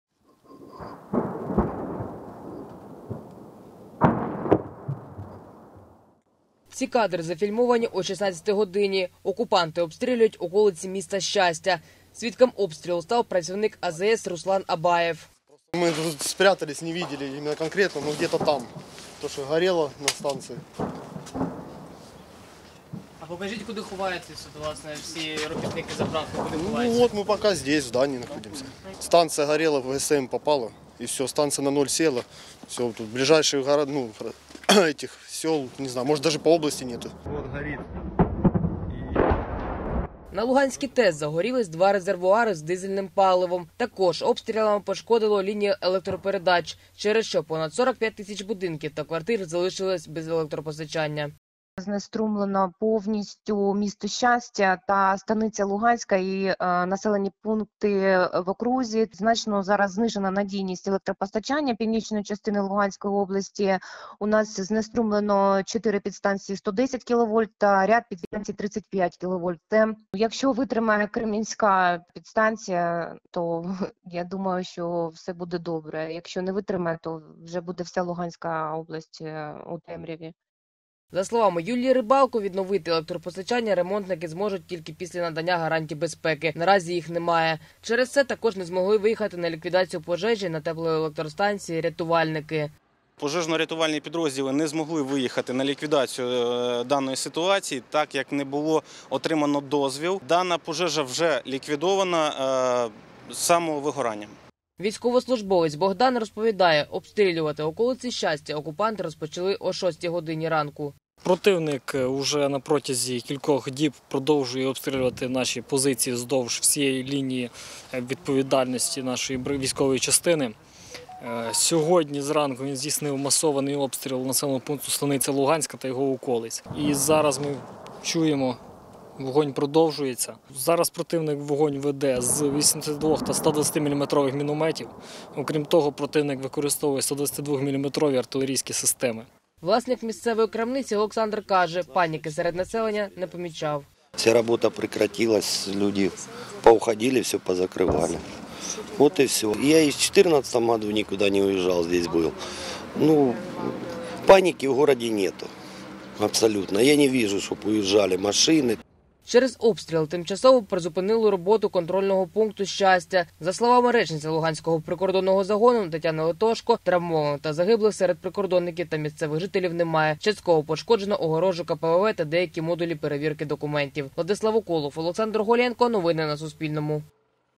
Жителі прифронтового Щастя залишаються без світла, води та тепла
Жителі прифронтового Щастя залишаються без світла, води та тепла. Сьогодні там зупинили Луганську теплову електростанцію. Внаслідок обстрілів біля складу паливно-мастильних матеріалів сталося  займання. Пожежу загасили, серед працівників загиблих та травмованих немає. Повідомляє керівник Щастинської міської ВЦА Олександр Дунець.